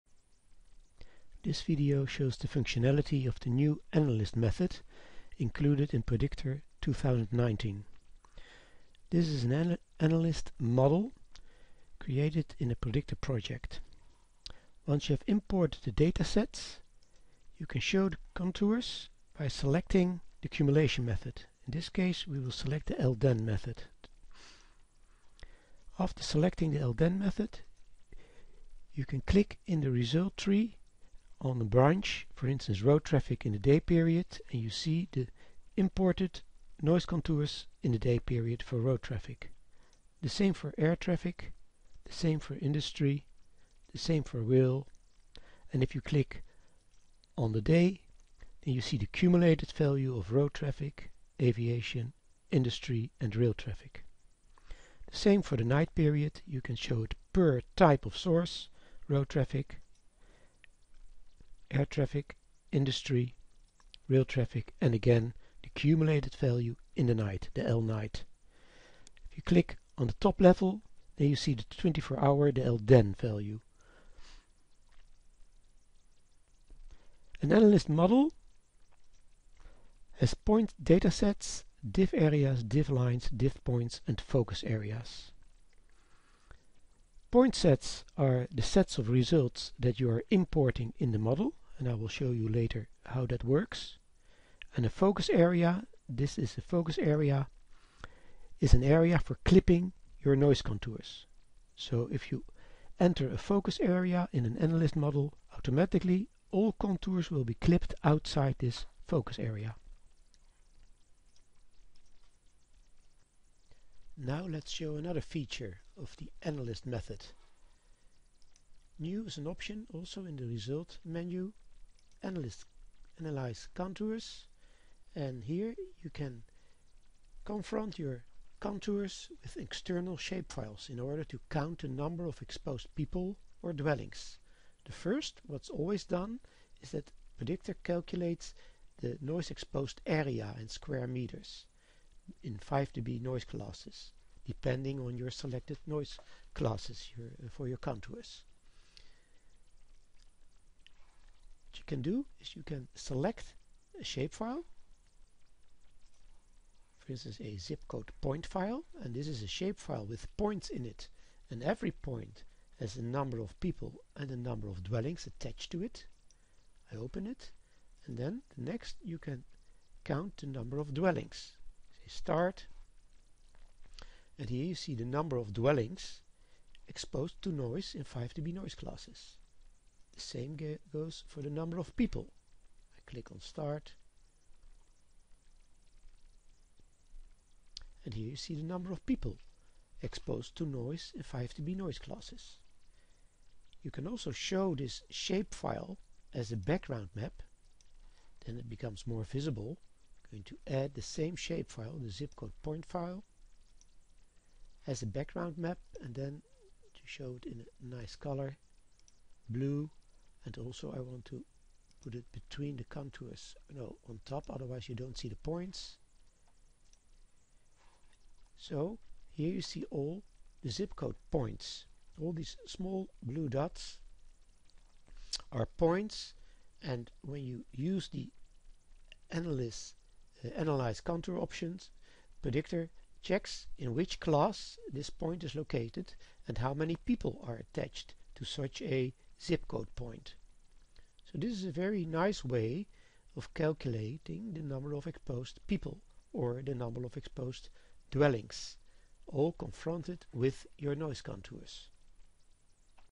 Predictor LimA Analyst Demo November 2018
Demonstrating the capabilities of the software as of late 2018.